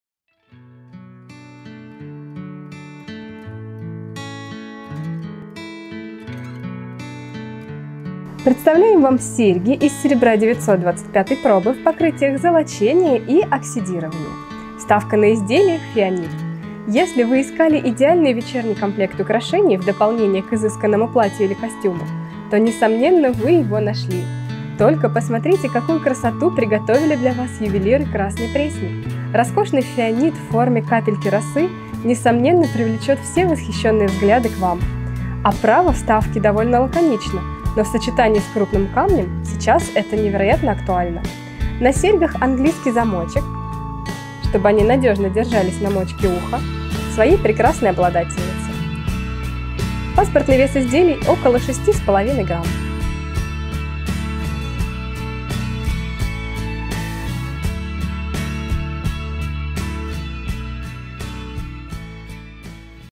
Серьги 33811759 (золочение) и Серьги 33811758 (оксидирование)
Вставка: фианит

Если вы искали идеальный вечерний комплект украшений в дополнение к изысканному платью или костюму, то несомненно - вы его нашли! Только посмотрите, какую красоту приготовили для Вас ювелиры Красной Пресни! Роскошный фианит в форме капельки росы - несомненно привлечет все восхищенные взгляды к вам. Оправа вставки довольно лаконична -  в сочетании с крупным камнем - сейчас это невероятно актуально! На серьгах английский замочек, чтобы они надёжно держались на мочке уха своей прекрасной обладательницы.

Паспортный вес изделий - около 6,5 грамм